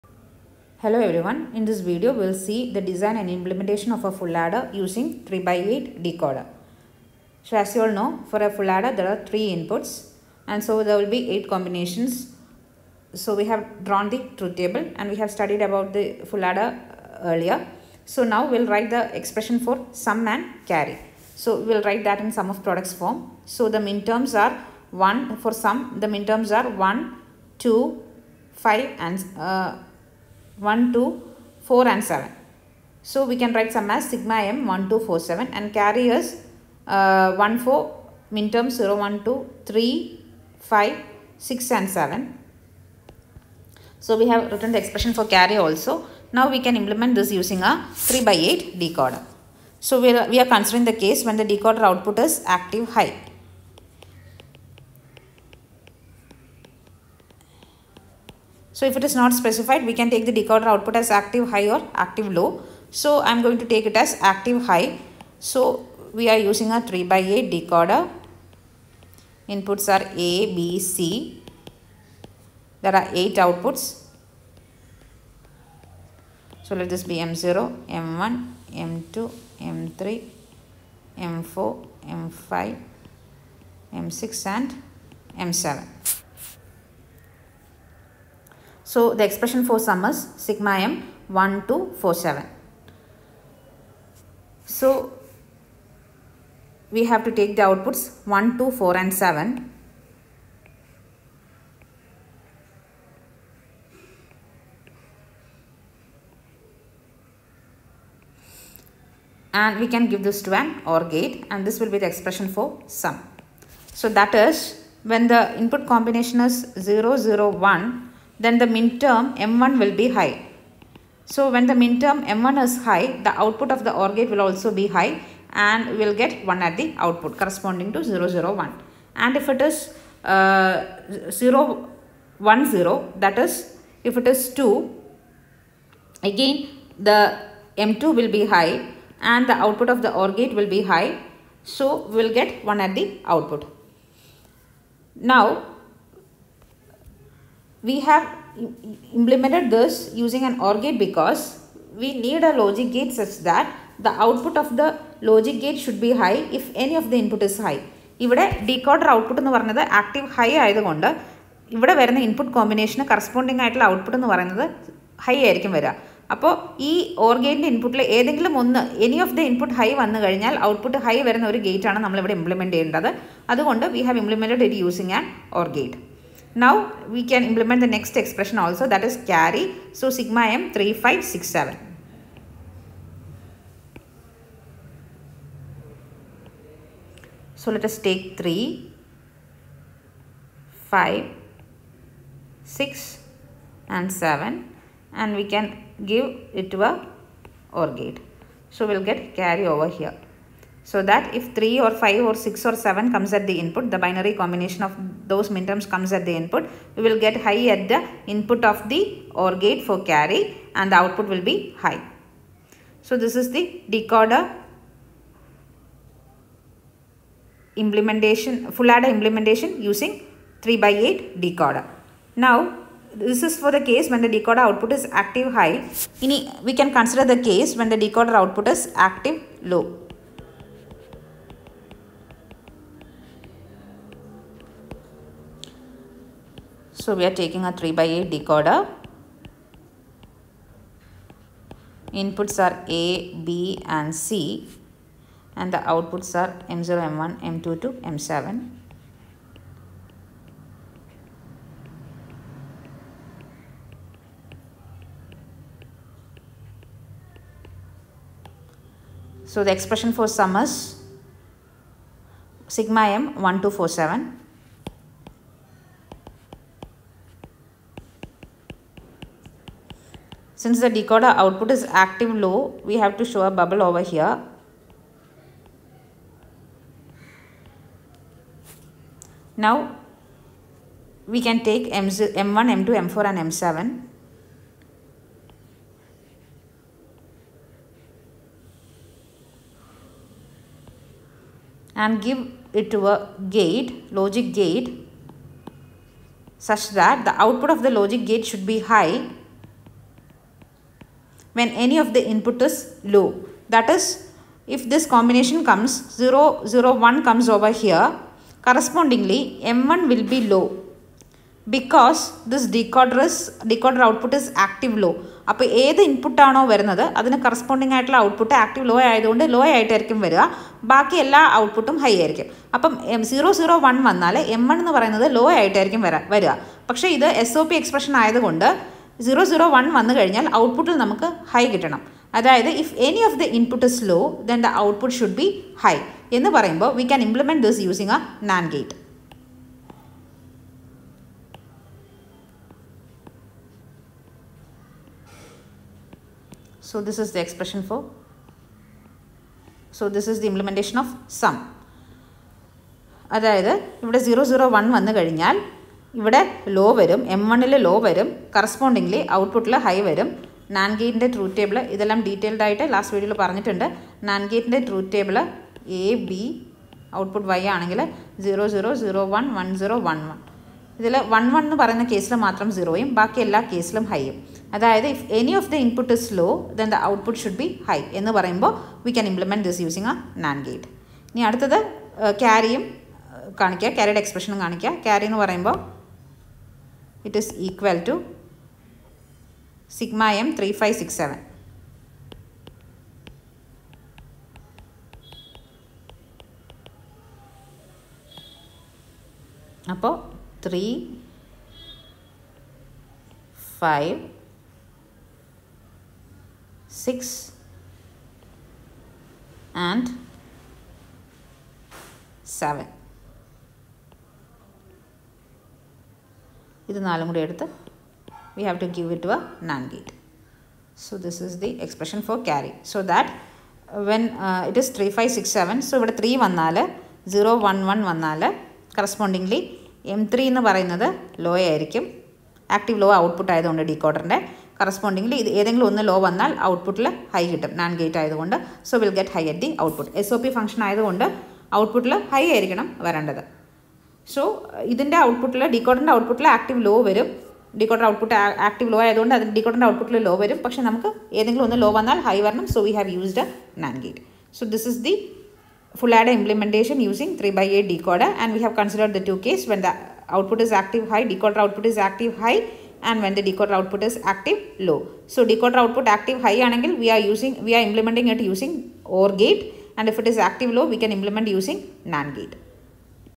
lecture 6: Design and implement full adder using decoder |KTU| Digital Electronics
Combinational circuit design using decoder